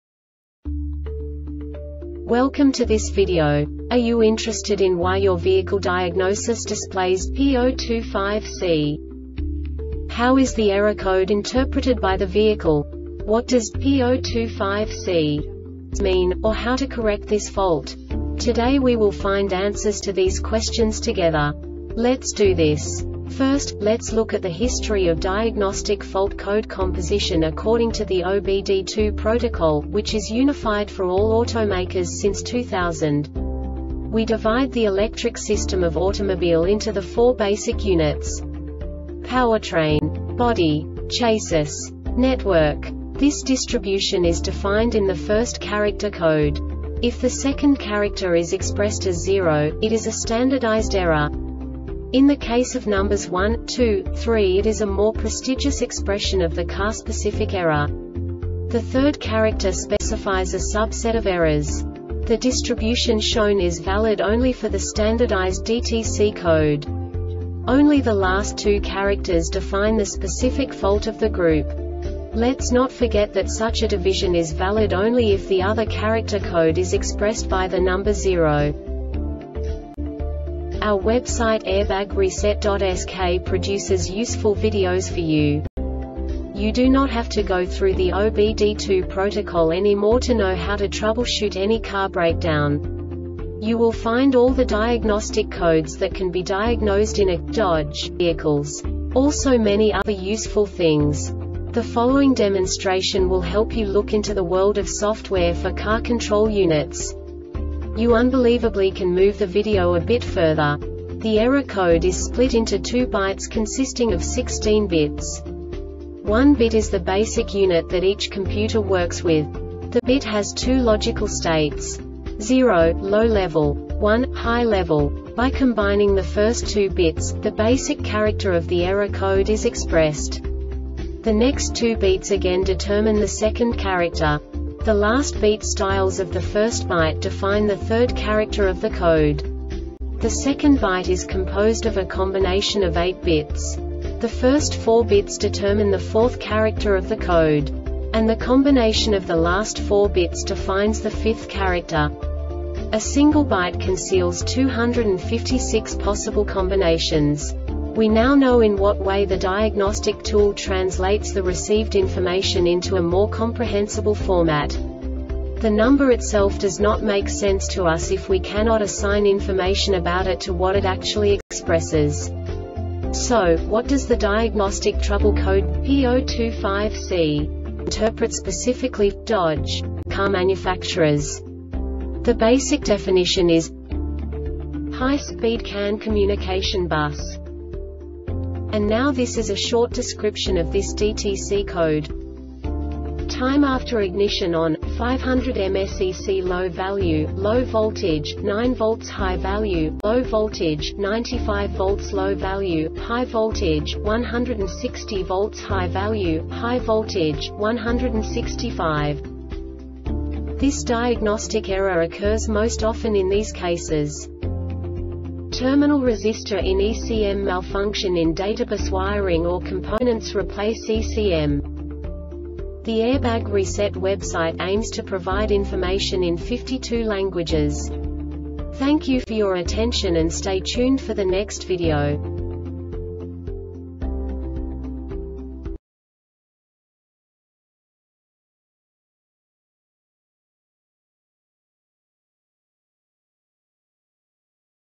DTC Dodge P025C Short Explanation
Contents:
0:21 Basic DTC analysis according to OBD2 protocol standard.
1:48 Insight into programming
2:58 A diagnostically understandable description of the Diagnostic Trouble Code
3:05 Basic error definition

Make
Dodge
Code
P025C
Definition
High Speed CAN Communication Bus
Description